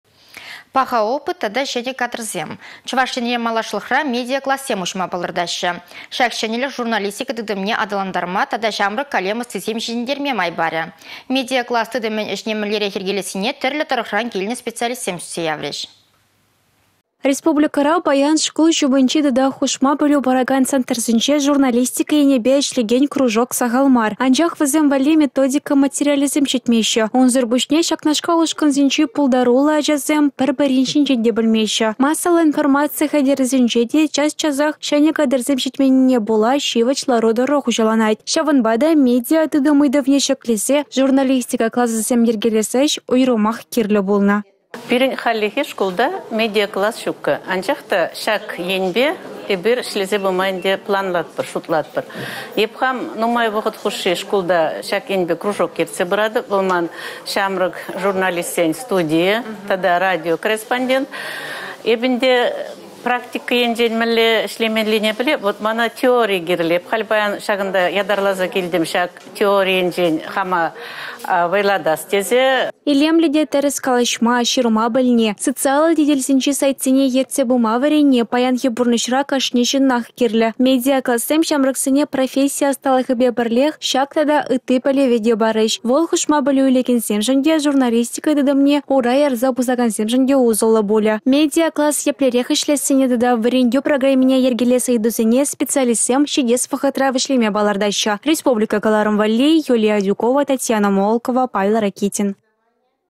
Чăваш Енре медиа-классем пулĕç
Паха опыт тата çĕнĕ кадрсем...  Чăваш Енре малашлăхра  медиа-классем уçма палăртаççĕ. Çак çĕнĕлĕх журналистика тытăмне аталантарма тата  çамрăк калем ăстисем çитĕнтерме май парĕ. Медиа-класс тытăмĕн ĕçне мĕнлерех йĕркелессине тĕрлĕ тăрăхран килнĕ специалистем сӳтсе яврĕç.